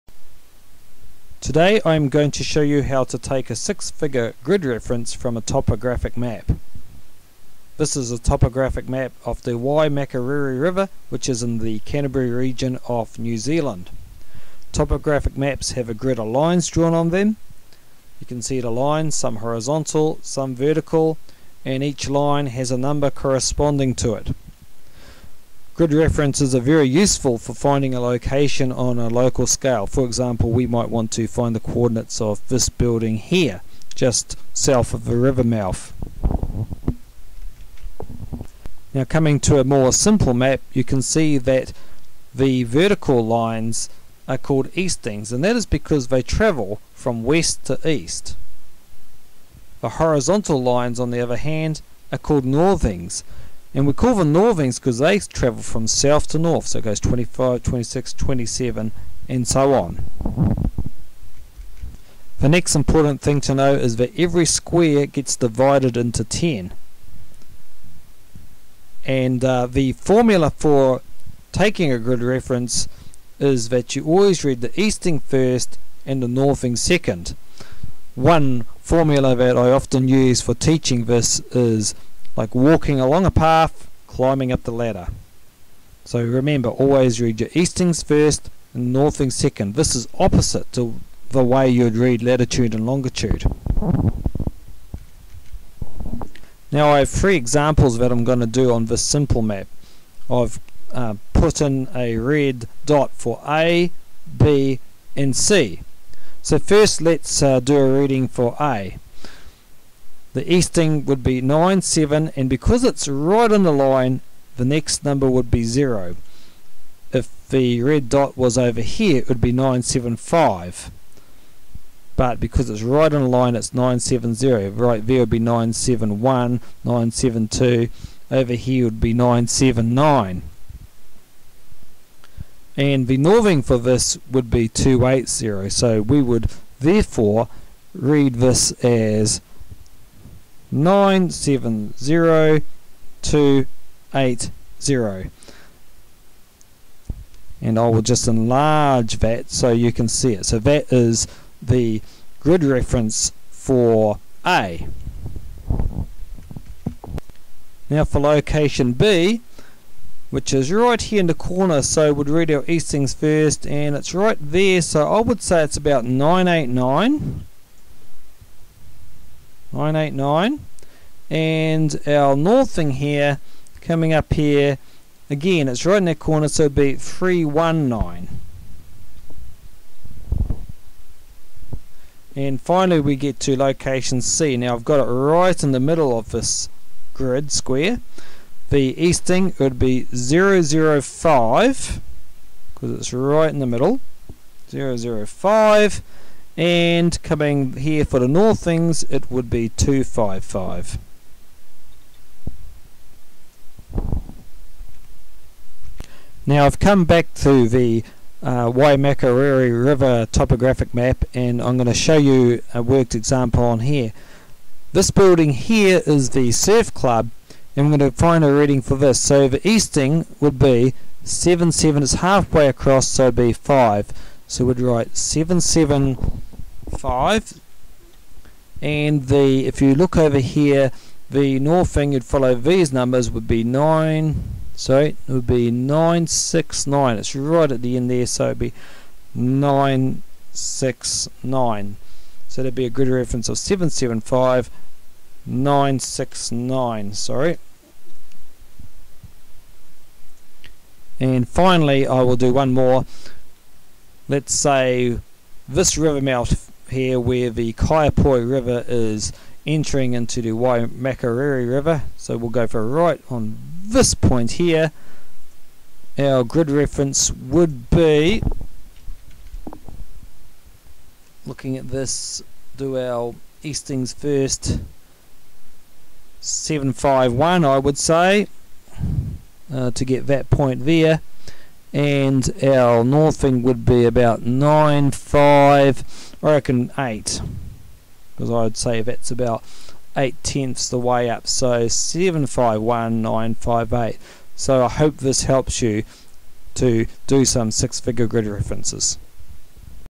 How to read a 6 figure Grid Reference
How to read a 6 figure Grid Reference from a Topographic Map. Grid references are useful for finding a location on a local scale. The vertical lines are called eastings and the horizontal lines are called northings. Eastings are read first, northings second.
The map is Sourced from NZ Topo Database. Crown Copyright Reserved.